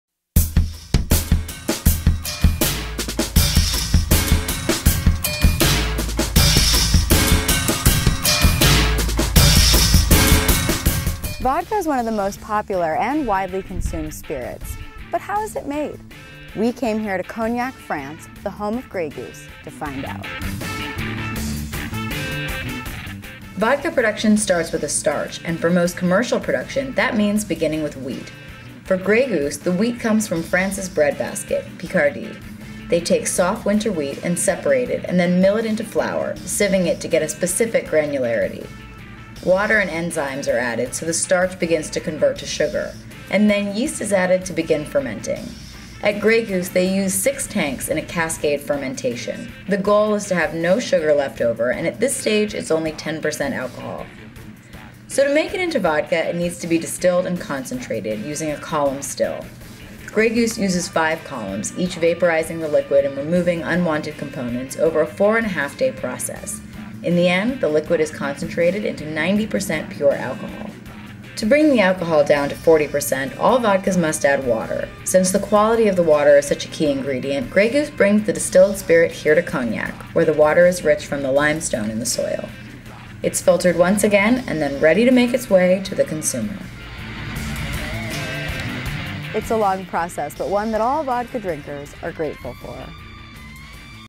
How Vodka is Made: A Trip to Grey Goose | Potluck Video
How Vodka is Made

Vodka is one of the world's most widely consumed spirits, but how is it made? We went to Cognac, France - the home of Grey Goose Vodka - to find out